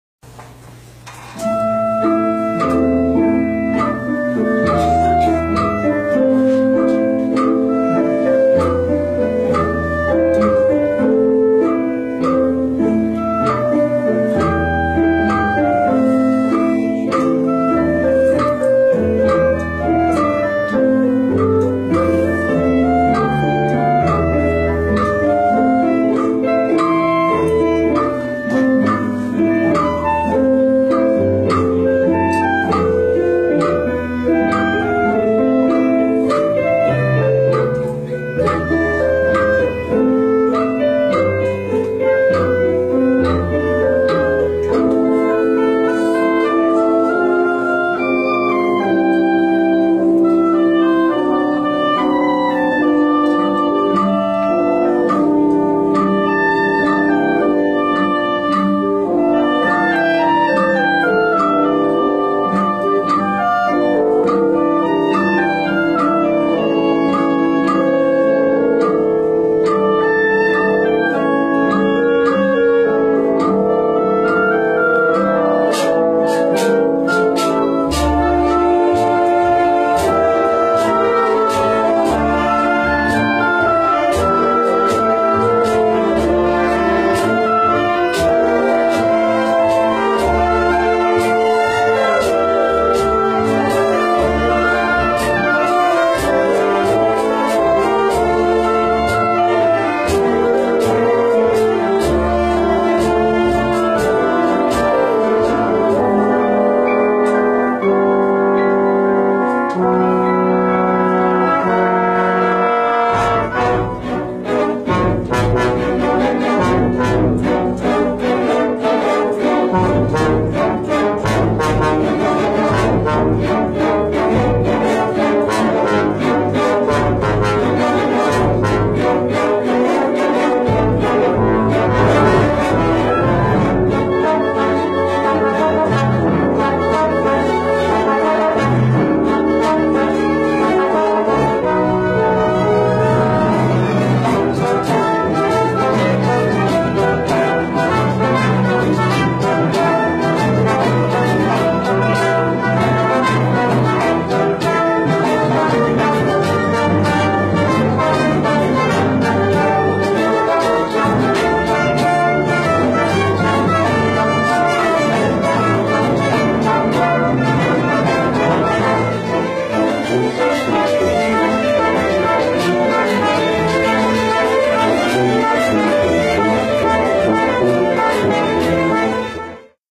Ensayos - Concierto Orquesta Filarmónica de Sonora
Presentación de la Orquesta Filarmónica de Sonora, el día 09 de octubre de 2015, 7:00 p.m., en el auditorio James K. Clark de Nogales High School, en el marco de la celebración del 130 Aniversario del Consulado General de México en Nogales, Arizona.